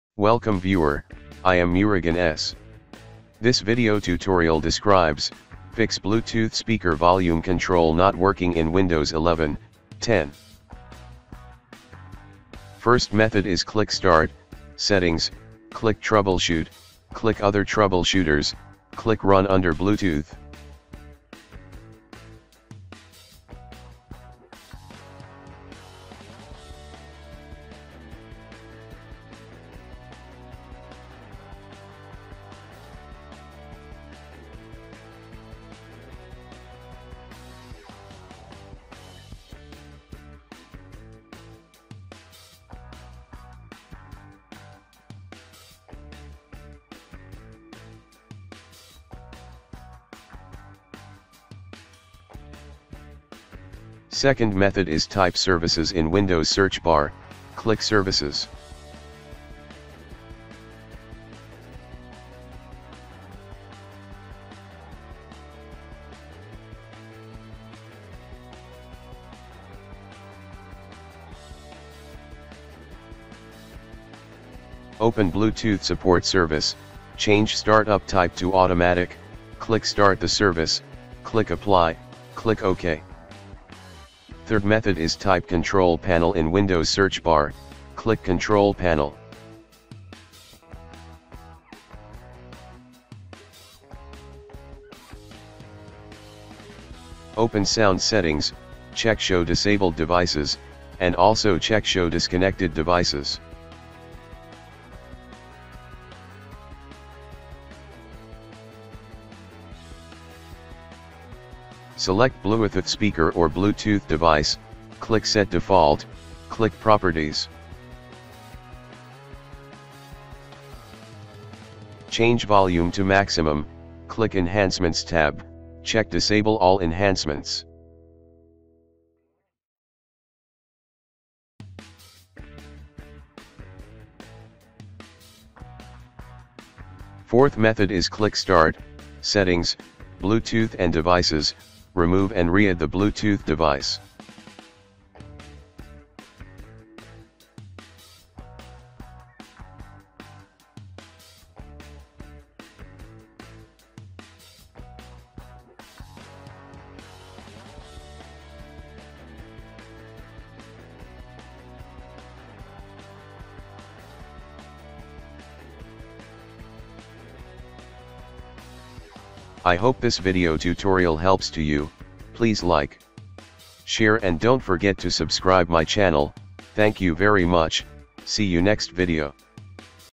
Fix Bluetooth Speaker Volume Control not Working In Windows 11/10
This Tutorial Helps to Fix Bluetooth Speaker Volume Control not Working In Windows 11/10

00:00 Intro
00:12 Method 1 - Troubleshooting
00:52 Method 2 - Change Service Type
01:24 Method 3 - Change Sound Settings
02:17 Method 4 - Re-Add Bluetooth Device
02:49 Closing